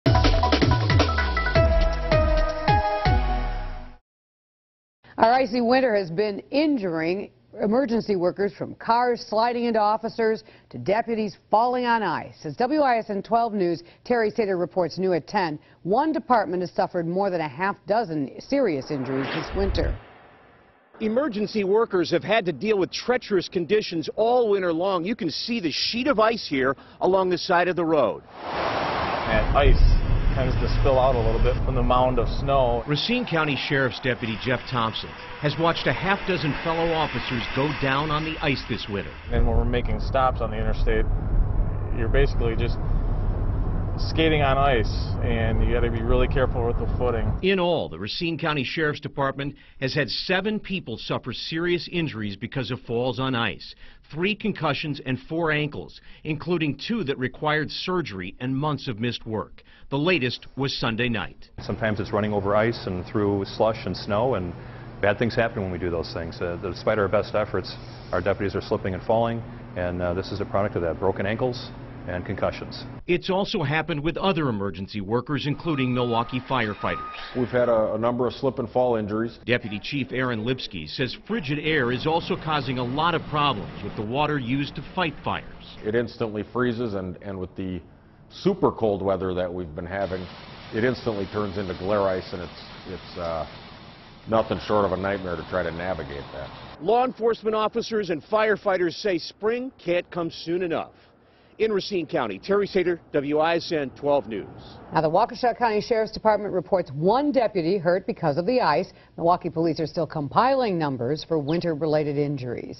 Icy winter causing injury for some emergency workers
12 News Terry Sater reports.